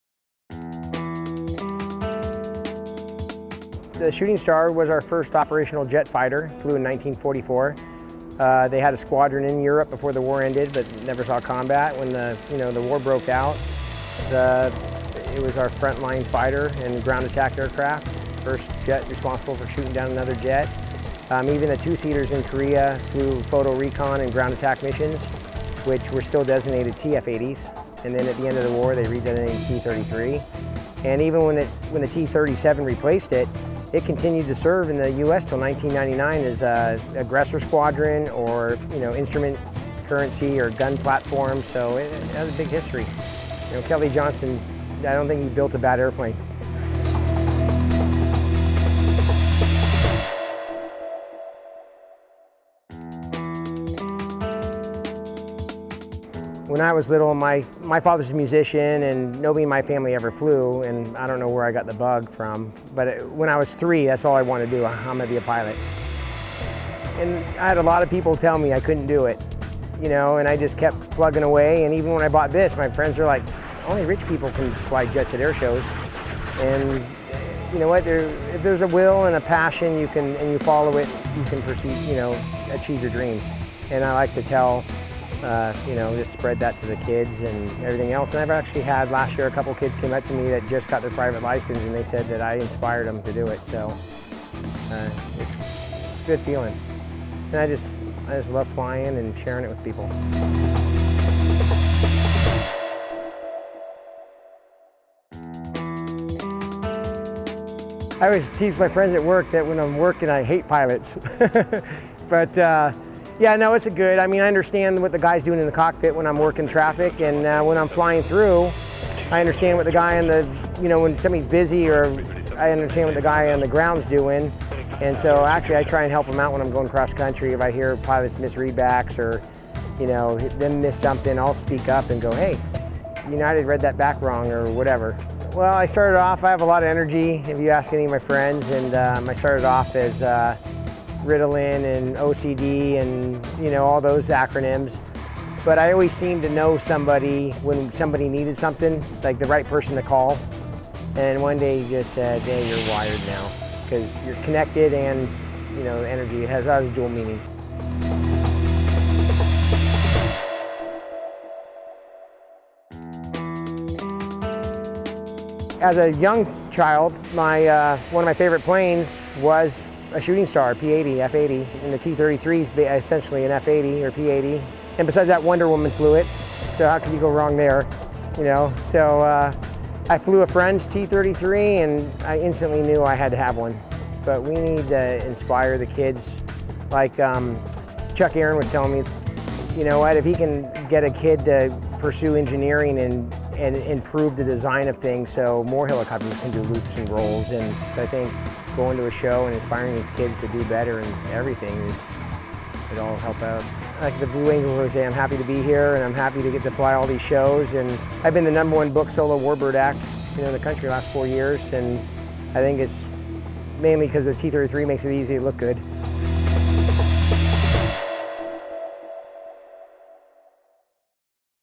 Pilot Logbook - Greg Colyer, "Acemaker" Lockheed T-33 Shooting Star
Pilot Greg "Wired" Colyer talks about the history of the Lockheed T-33 Shooting Star.  Colyer talks about how he became to be a pilot, what it's like flying the aircraft and sharing it with people.